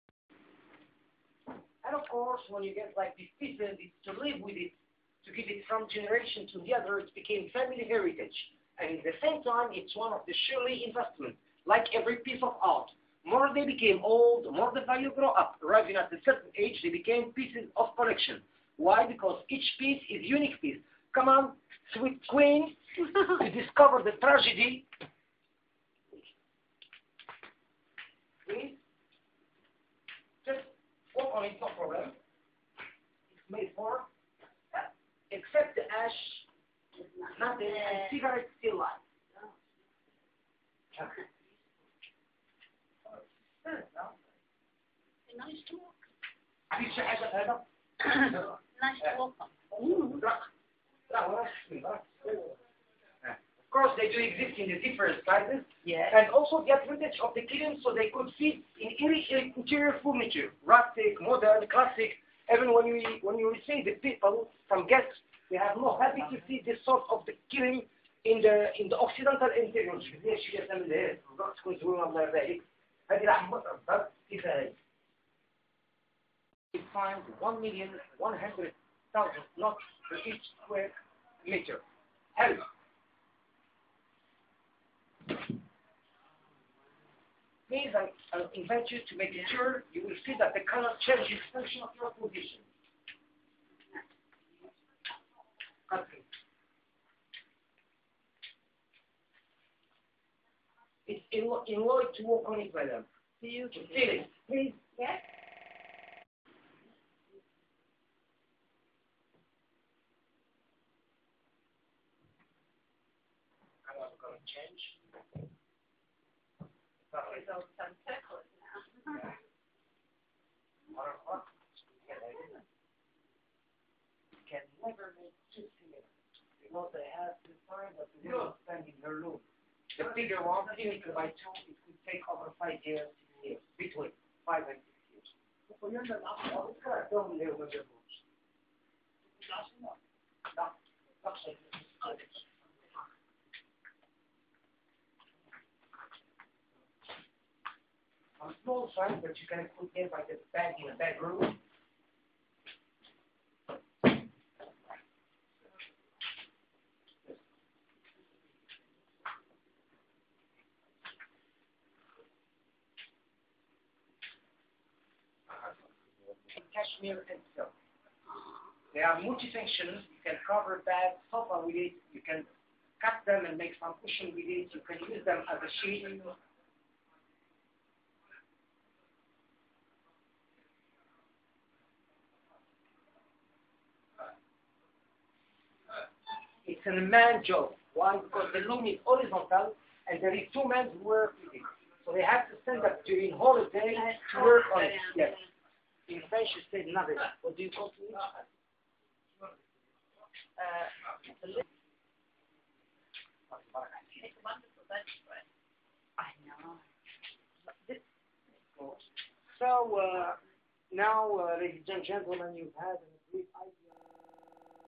The Berber Carpet Co-Operative - Tétouan, Morocco
A visit to the Berber Carpet Co-Operative in Tétouan, Morocco.  Probably this video is only for carpet and rug enthusiasts, but the opening scene is quite interesting because it demonstrates how a burning cigarette, left in a folded carpet, does no harm.  There is a separate video showing the Royal Palace and the Medina.